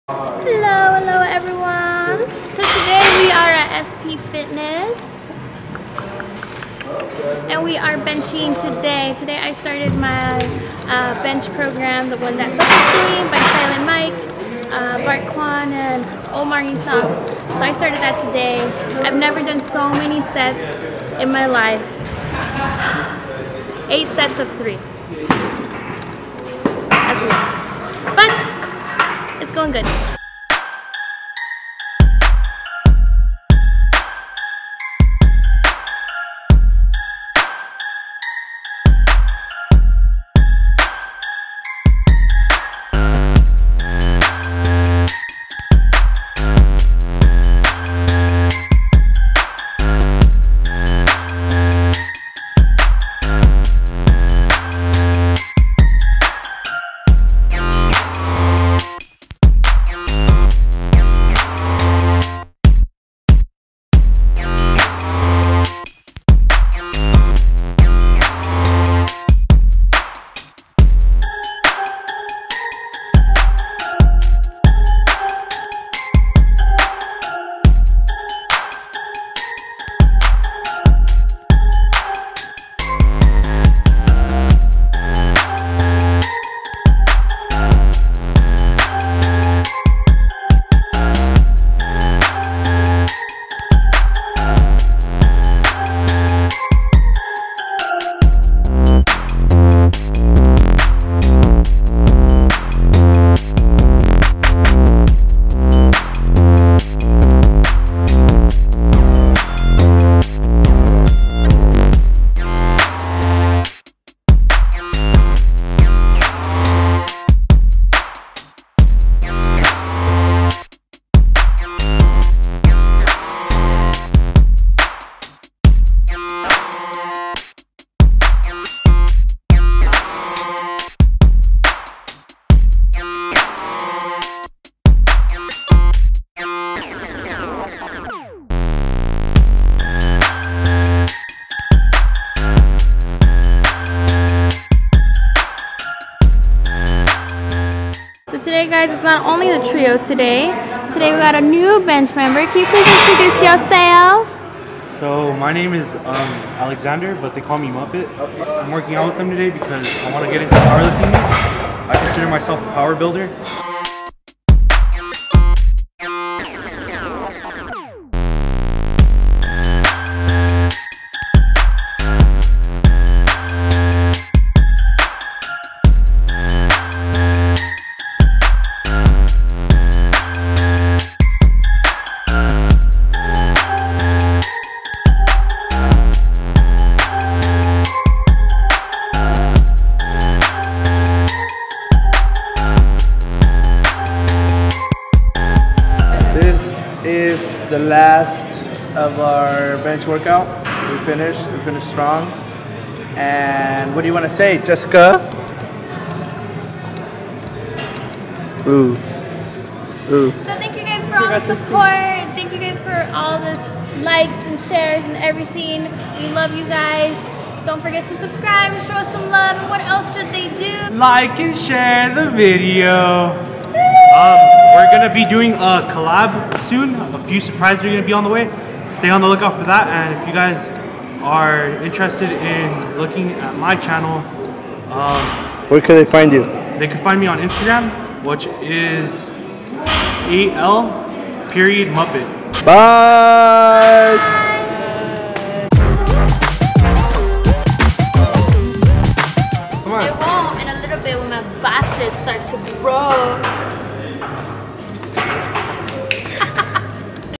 STARTING A NEW BENCH PROGRAM
We are back in Santa Paula and back at SP fitness to workout on the chest. But its not just the trio, today Alexander joined our bench day. We met him here at Sp fitness. He's an aspiring powerlifter and youtuber at only 15 years young.